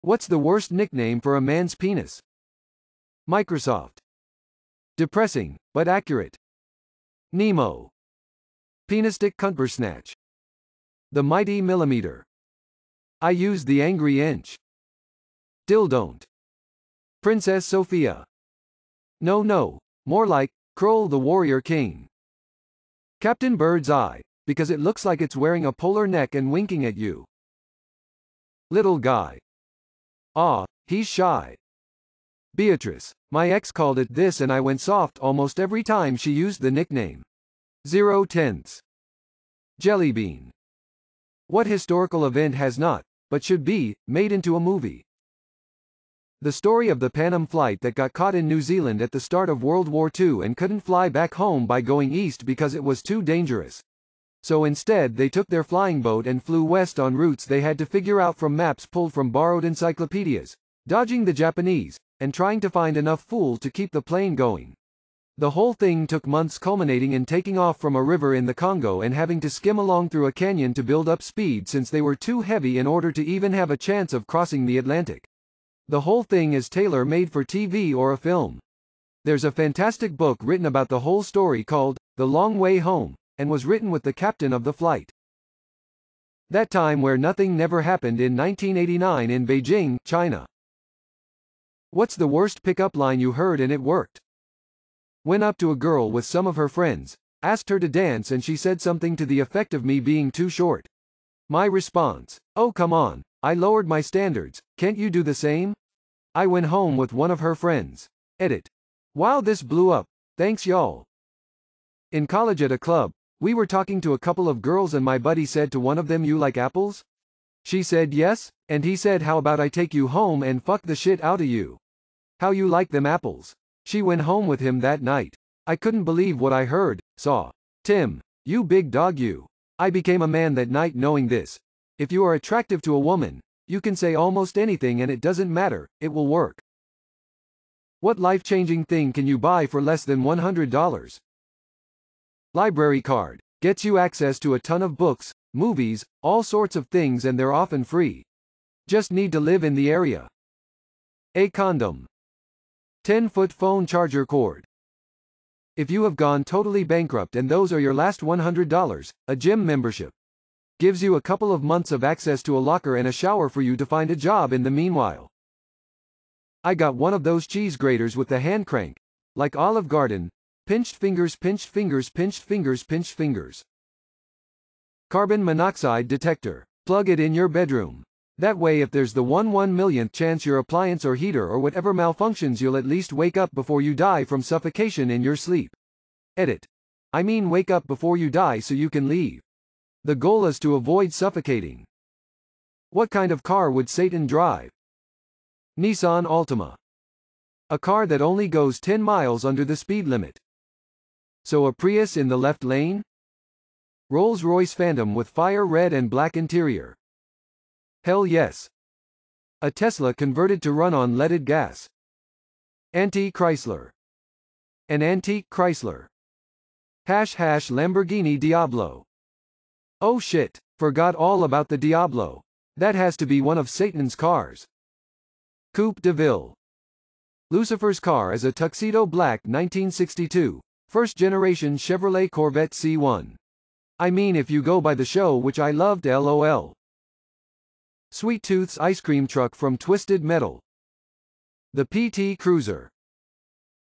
AskReddit Compilation #248
Featuring:
What's the worst nickname for a man's penis?
What historical event has not, but should be, made into a movie?
What's the worst pick up line you heard and it worked?
What life changing thing can you buy for less than $100?
What kind of car would Satan drive?

💬 Comment your story!